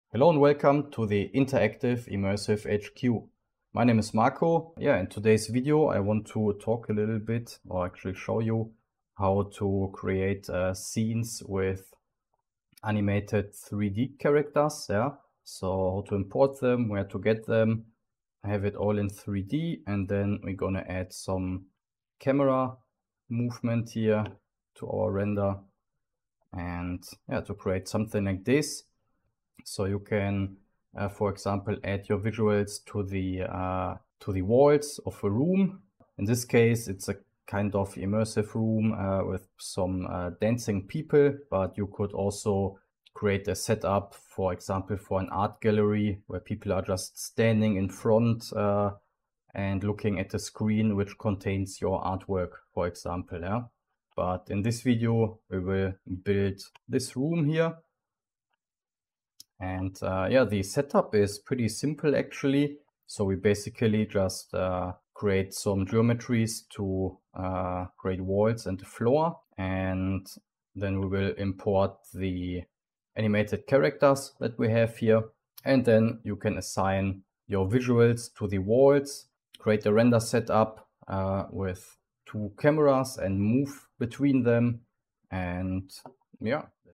Using 3D Models with Animations in TouchDesigner
We look at how to import and use animated 3D models in TouchDesigner.

This technique lets you present your visuals in a new context and is perfect for simulations, virtual environments, or giving your work a fresh, dynamic setting.